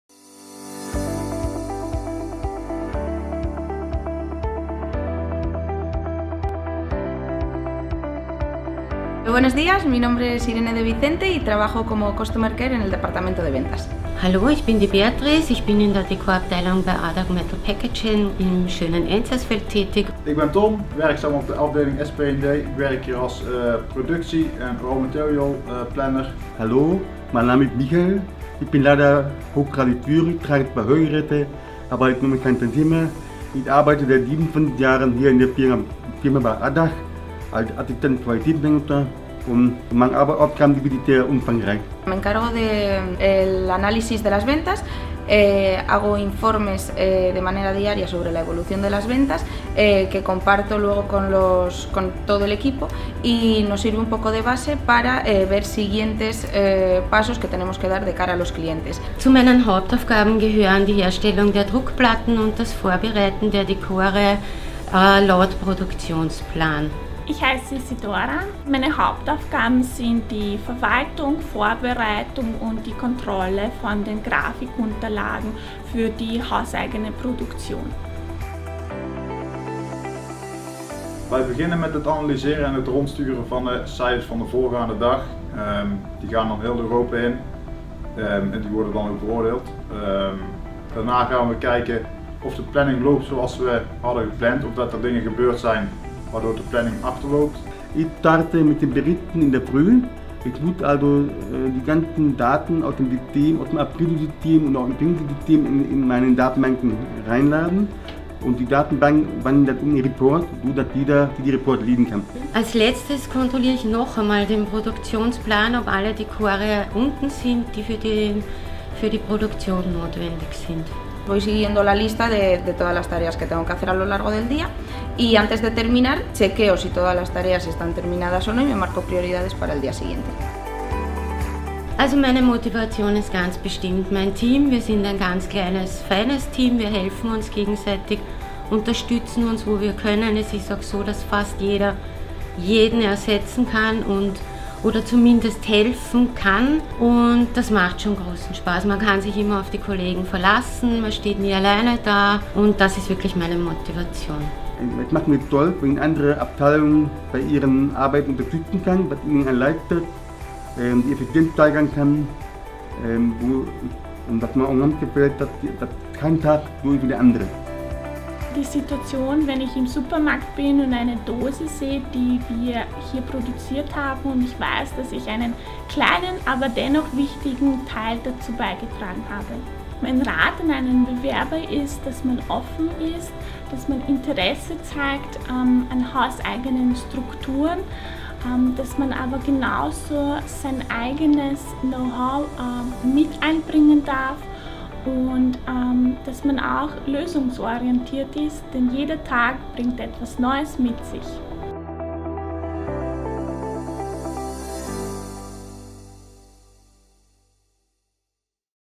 Ogłoszenie o pracę w AMP - Europe - stanowiska biurowe w zakładach
W tym krótkim wideo dowiedz się więcej o naszych zakresach obowiązków na stanowiskach biurowych w zakładach - cieszymy się na spotkanie z Tobą!